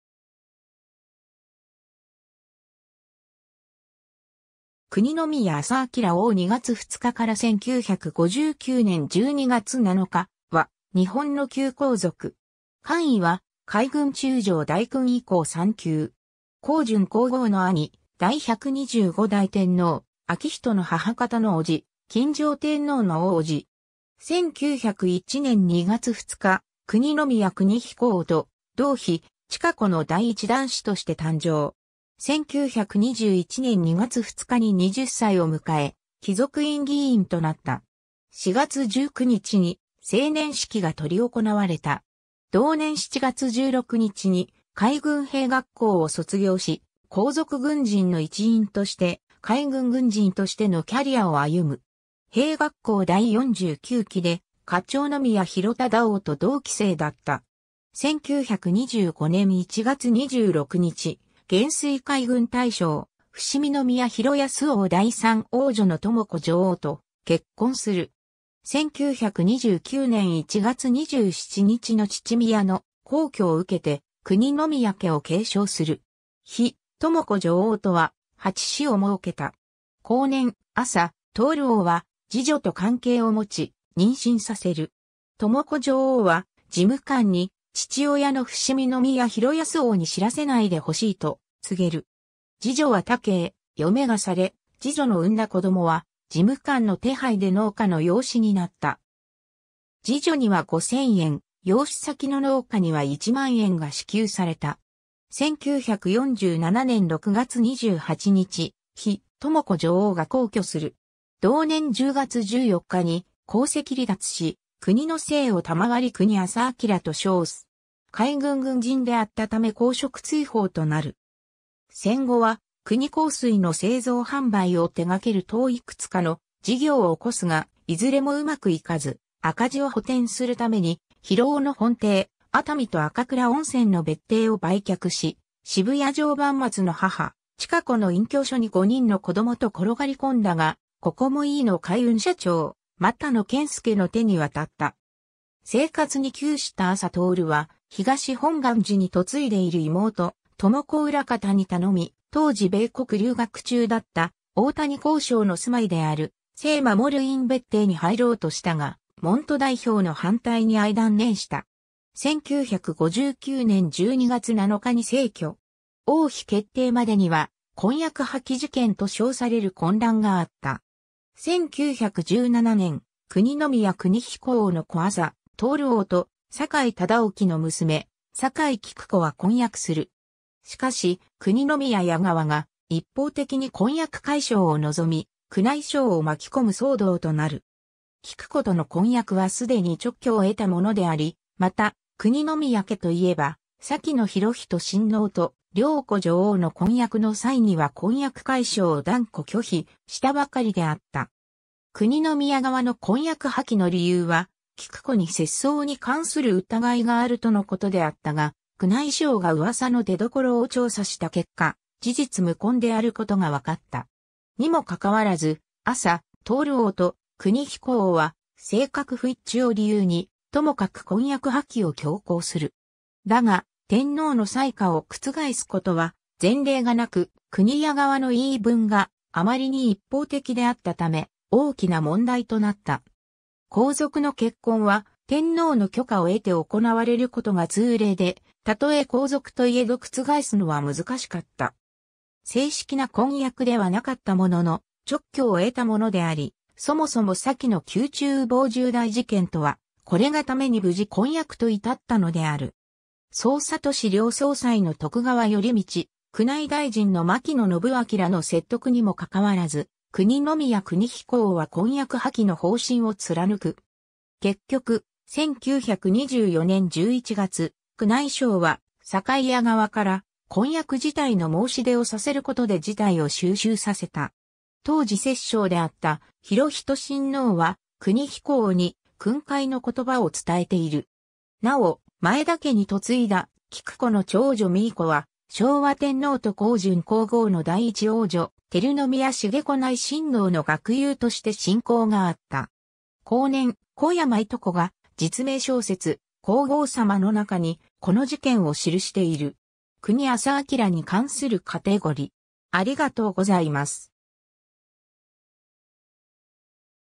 久邇宮朝融王
日本の王_(皇族)
久邇宮朝融王（くにのみや あさあきらおう、1901年（明治34年）2月2日 - 1959年（昭和34年）12月7日）は、日本の旧皇族。
官位は海軍中将大勲位功三級。
香淳皇后の兄、第125代天皇明仁の母方の伯父、今上天皇の大伯父。
 1901年（明治34年）2月2日、久邇宮邦彦王と同妃俔子の第1男子として誕生。
 1921年（大正10年）2月2日に20歳を迎え、貴族院議員となった。
4月19日に、成年式が執り行われた。
 同年7月16日に海軍兵学校を卒業し、皇族軍人の一員として海軍軍人としてのキャリアを歩む。
兵学校第49期で、華頂宮博忠王と同期生だった。
 1925年（大正14年）1月26日、元帥海軍大将伏見宮博恭王第3王女の知子女王と結婚する。
1929年（昭和4年）1月27日の父宮の薨去を受けて久邇宮家を継承する。
妃知子女王とは、8子を儲けた。
 後年、朝融王は侍女と関係を持ち、妊娠させる。
知子女王は、事務官に父親の伏見宮博恭王に知らせないでほしいと告げる。
侍女は他家へ嫁がされ、侍女の産んだ子供は、事務官の手配で農家の養子になった。
侍女には5000円、養子先の農家には1万円が支給された（当時の総理大臣の年俸は1万円に満たない額だった）。
 1947年（昭和22年）6月28日、妃知子女王が薨去する。
同年10月14日に皇籍離脱し、久邇の姓を賜り久邇朝融と称す。
海軍軍人であったため公職追放となる（1952年解除）。
 戦後は『久邇香水』の製造販売を手掛ける等幾つかの事業を興すがいずれも上手くいかず、赤字を補填するために広尾の本邸（現・聖心女子大学「パレス」）、熱海と赤倉温泉の別邸を売却し、渋谷常磐松の母・俔子の隠居所に5人の子供と転がり込んだが、ここも飯野海運社長・俣野健輔の手に渡った。
 生活に窮した朝融は、東本願寺に嫁いでいる妹・智子裏方に頼み、当時米国留学中だった大谷光紹の住まいである成護院別邸に入ろうとしたが、門徒代表の反対に遇い断念した 。
 1959年（昭和34年）12月7日に逝去。
 王妃決定までには『婚約破棄事件』と称される混乱があった。
1917年（大正6年）、久邇宮邦彦王の子朝融王と酒井忠興の娘酒井菊子は婚約する。
しかし、久邇宮家側が一方的に婚約解消を望み、宮内省をまきこむ騒動となる。
菊子との婚約は既に勅許を得たものであり、また久邇宮家と言えば先の裕仁親王（のちの昭和天皇）と良子女王の婚約の際には婚約解消を断固拒否したばかりであった（これを宮中某重大事件という）。
 久邇宮側の婚約破棄の理由は「菊子に節操にかんする疑いがある」とのことであったが、宮内省が噂の出どころを調査した結果、事実無根であることが解った。
にもかかわらず朝融王と邦彦王は性格不一致を理由にともかく婚約破棄を強行する。
だが天皇の裁可を覆すことは前例がなく、久邇家側の言い分があまりに一方的であったため、大きな問題となった。
皇族の結婚は天皇の許可を得て行われることが通例で、たとえ皇族といえど覆すのは難しかった。
正式な婚約ではなかったものの、勅許を得たものであり、そもそも先の宮中某重大事件とはこれがために無事婚約と至ったのである。
宗秩寮総裁の徳川頼倫、宮内大臣の牧野伸顕らの説得にも関わらず、久邇宮邦彦王は婚約破棄の方針を貫く。
 結局、1924年（大正13年）11月、宮内省は酒井家側から婚約辞退の申し出をさせることで事態を収拾させた。
当時摂政であった裕仁親王（昭和天皇）は、邦彦王に訓戒の言葉を伝えている。
 なお、前田家に嫁いだ菊子の長女美意子は、昭和天皇と香淳皇后の第一皇女照宮成子内親王（朝融王から見て姪にあたる）の学友として親交があった。
 後年、小山いと子が実名小説『皇后さま』の中に、この事件を記している。
 久邇朝融に関するカテゴリ: